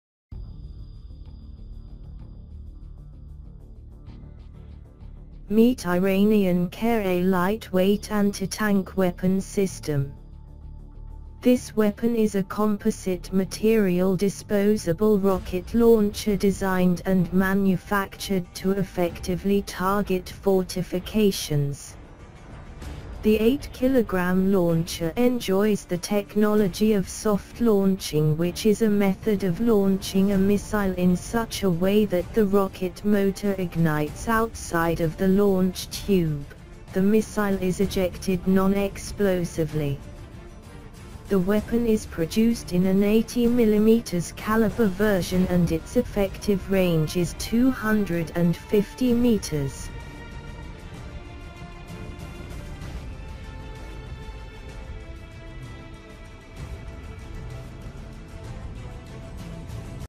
Iran recently unveiled Qare'a Light-Weight Anti-Tank Weapon System
Iran recently unveiled Iranian Qare'a Light-Weight Anti-Tank Weapon System.

This weapon is a Composite Material Disposable Rocket Launcher designed and manufactured to effectively target fortifications.

The 8-kilogram launcher enjoys the technology of soft launching which is a method of launching a missile in such a way that the rocket motor ignites outside of the launch tube; the missile is ejected non-explosively.

The weapon is produced in an 80mm calibre version and its effective range is 250 meters.